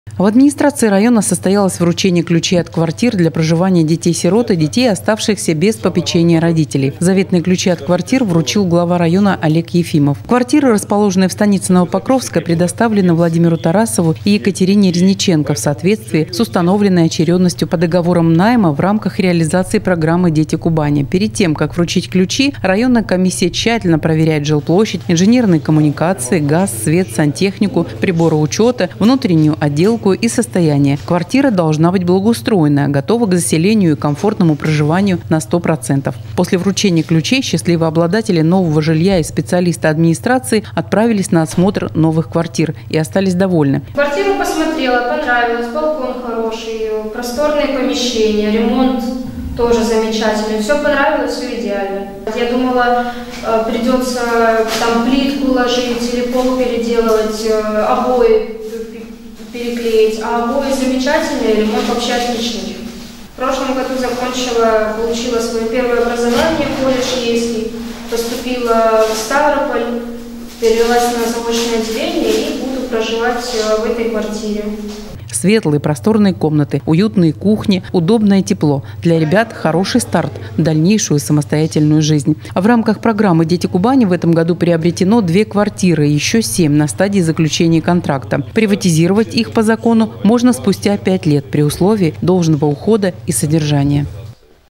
Ключи от квартир вручены новоселам
В администрации района состоялось вручение ключей от квартир для проживания детей-сирот и детей, оставшихся без попечения родителей. Заветные ключи от квартир вручил глава района Олег Ефимов.
Квартиры, расположенные станице Новопокровская, предоставлены Владимиру Тарасову и Екатерине Резниченко в соответствии с установленной очередностью по договорам найма в рамках реализации программы «Дети Кубани».
Перед тем как вручить ключи районная комиссия тщательно проверяет жилплощадь, инженерные коммуникации, газ, свет, сантехнику, приборы учета, внутреннею отделку и состояние. Квартира должна быть благоустроена, готова к заселению и к комфортному проживанию на 100 %.
После вручения ключей счастливые обладатели нового жилья и специалисты администрации отправились на осмотр новых квартир и остались довольны: светлые, просторные комнаты, уютные кухни, удобно и тепло – для ребят хороший старт в дальнейшую самостоятельную жизнь.
В рамках программы Дети Кубани» в этом году приобретено две квартиры, ещё семь на стадии заключения контракта. Приватизировать их по закону можно спустя 5 лет при условии должного ухода и содержания.